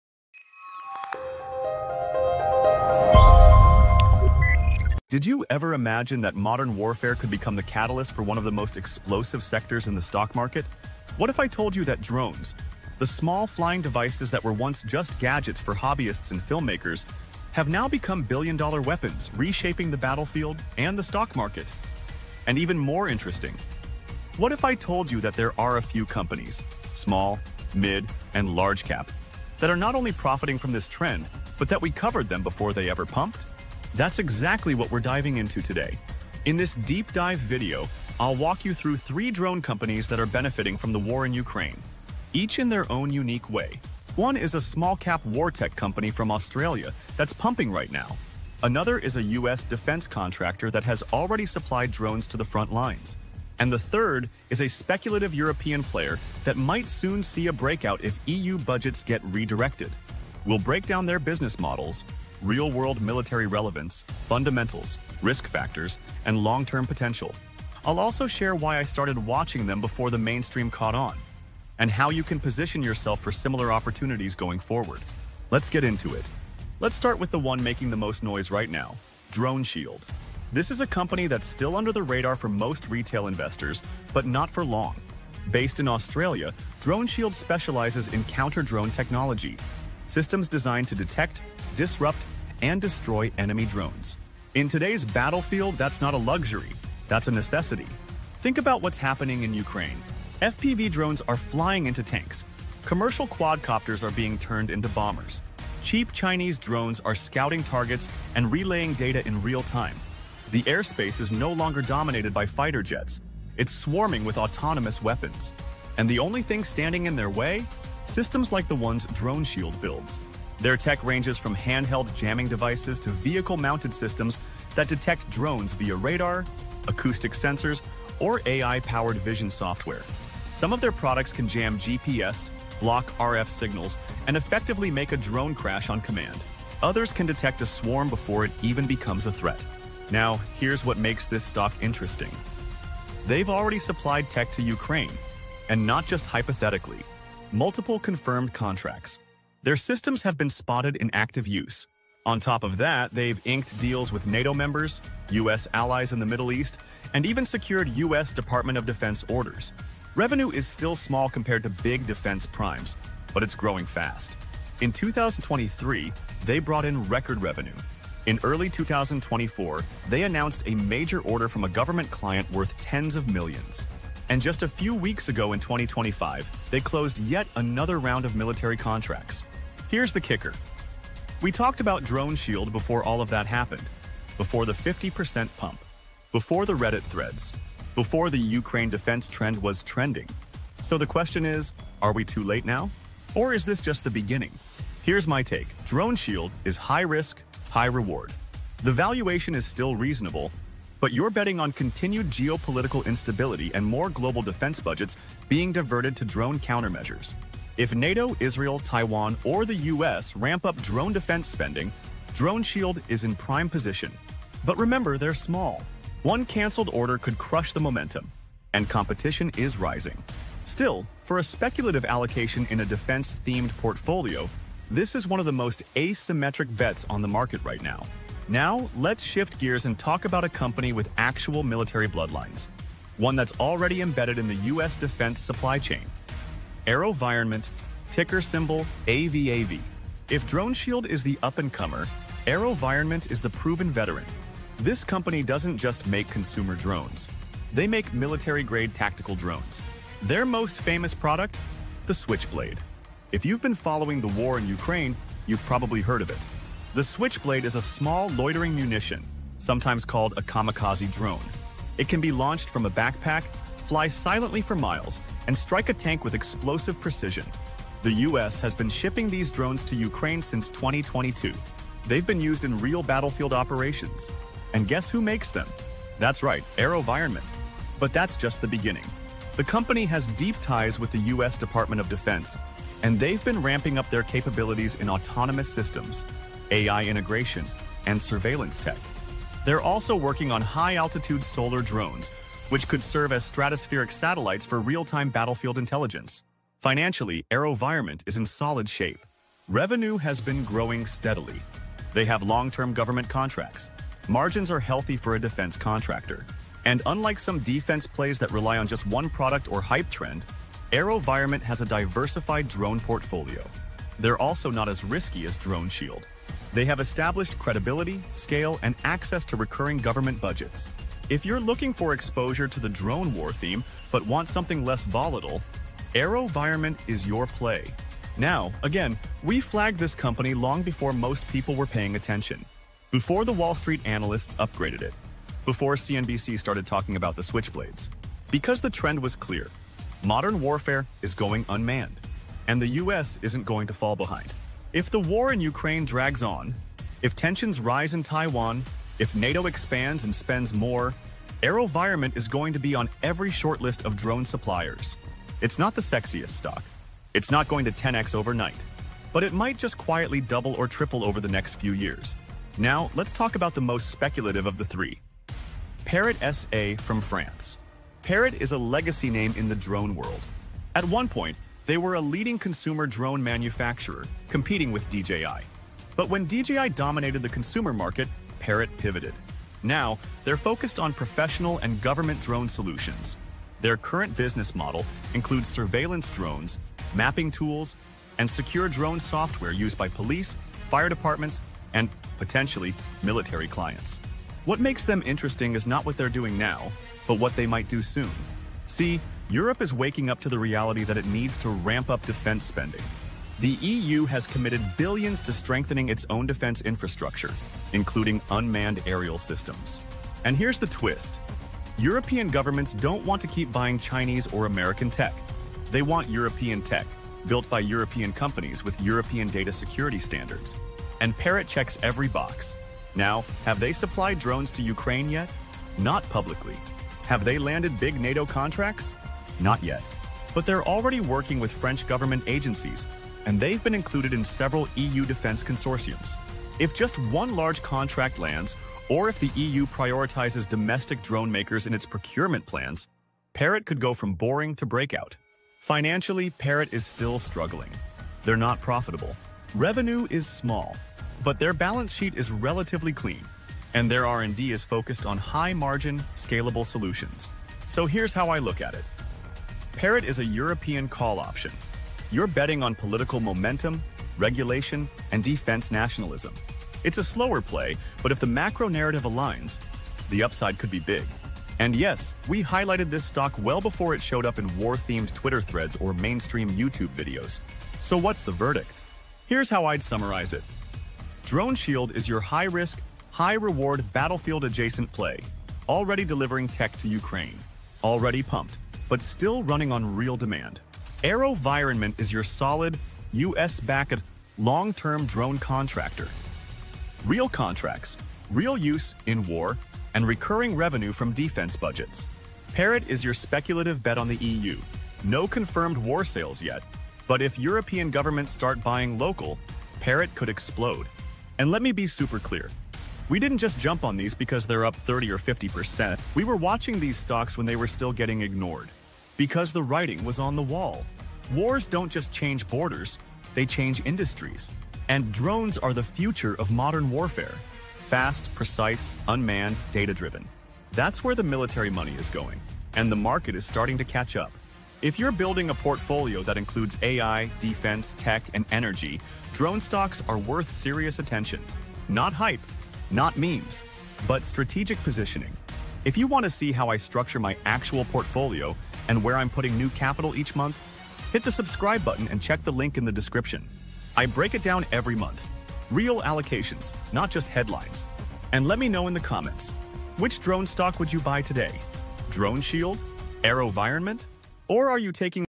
3 drone stocks that profit from the war
Ukraine Drone War: How AI Drones Are Changing the Battlefield

The war in Ukraine has become the ultimate testing ground for next-gen military drones.

From low-cost FPV kamikaze drones to AI-powered autonomous units, this is how technology is reshaping the future of warfare – in real time.

Join our free newsletter for weekly deep dives on finance, war tech & stock picks:
📩 [Coming soon – CapitalSuite Newsletter]